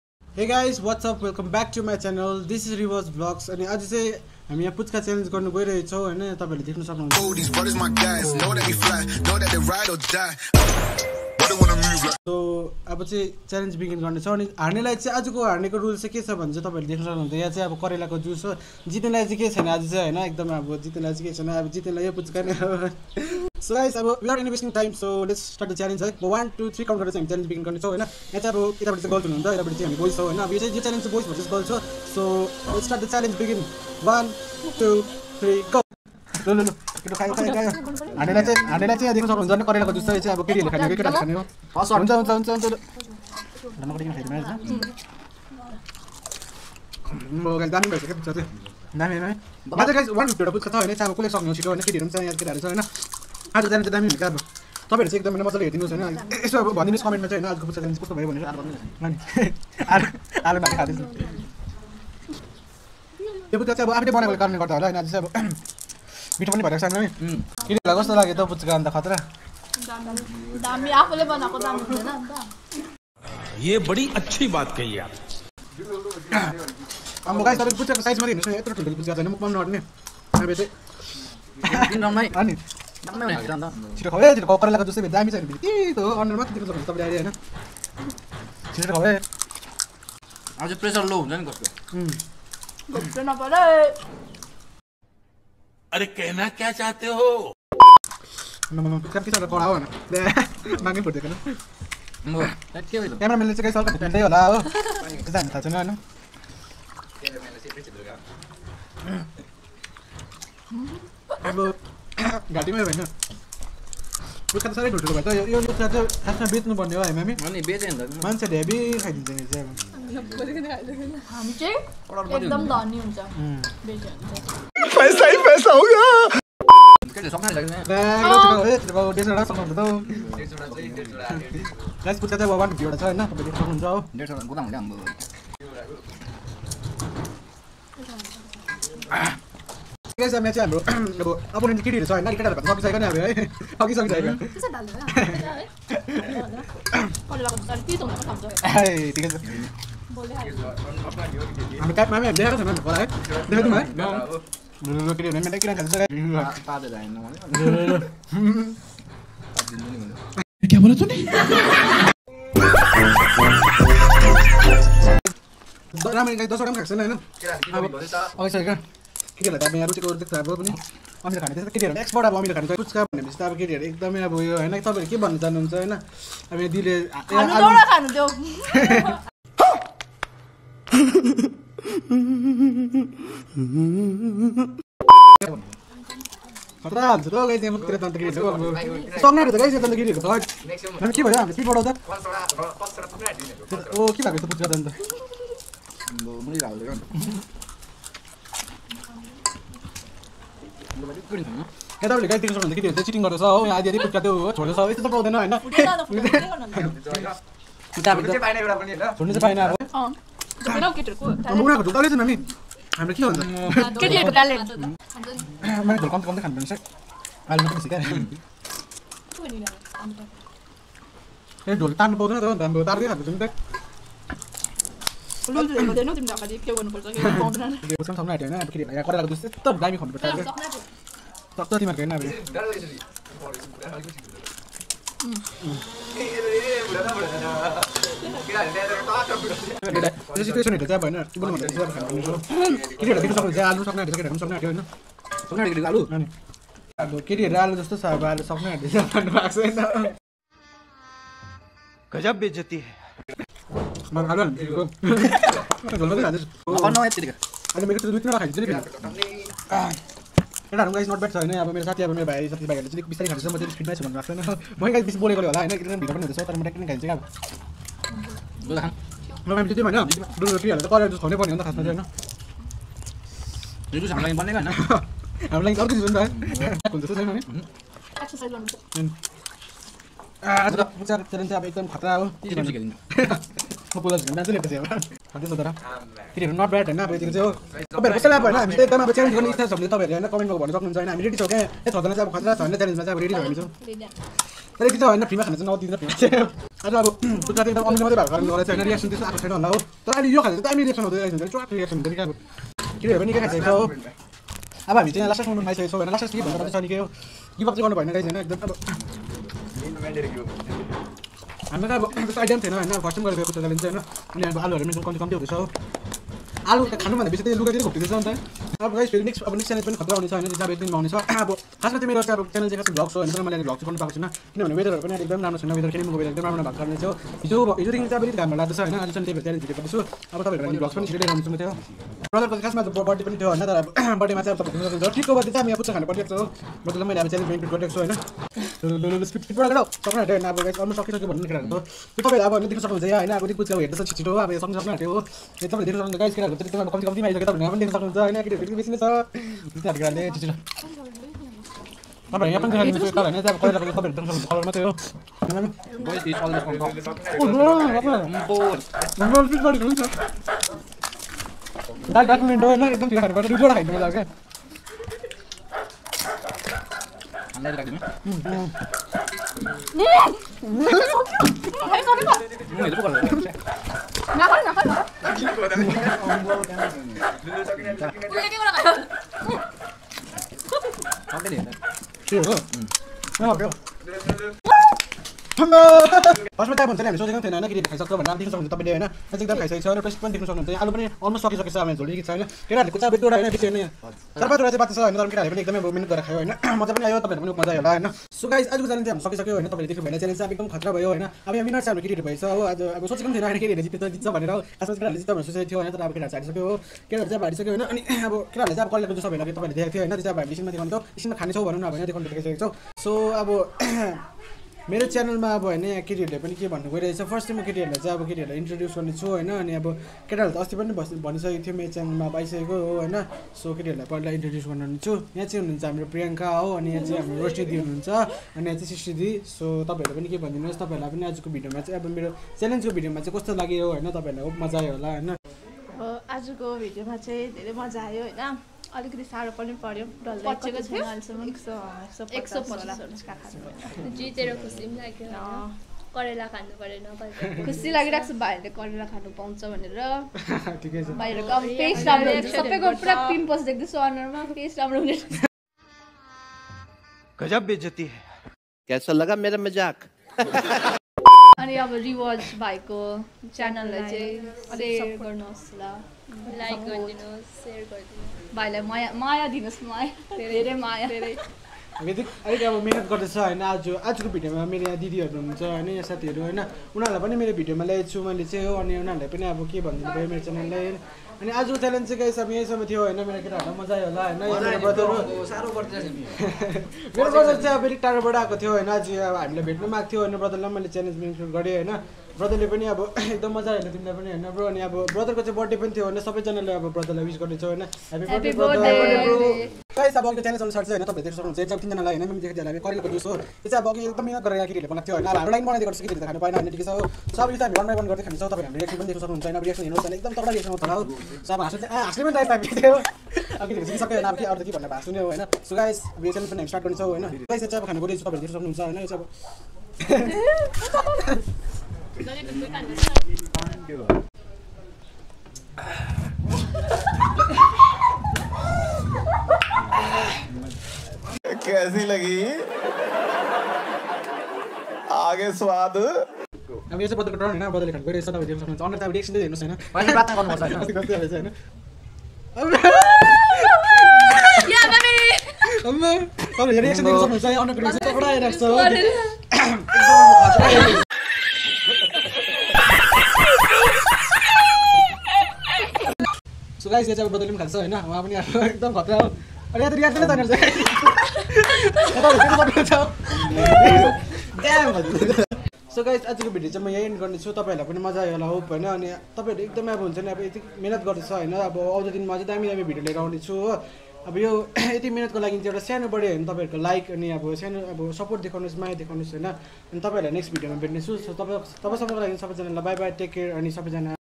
New video puchka challenge Girls vs Boys.✌️❤️ challenge goes very hard😵🔥 and fun watch in the vdeo!!
just focus in your goal and your dreams 😌❤️

stay safe stay healthy at home ❤️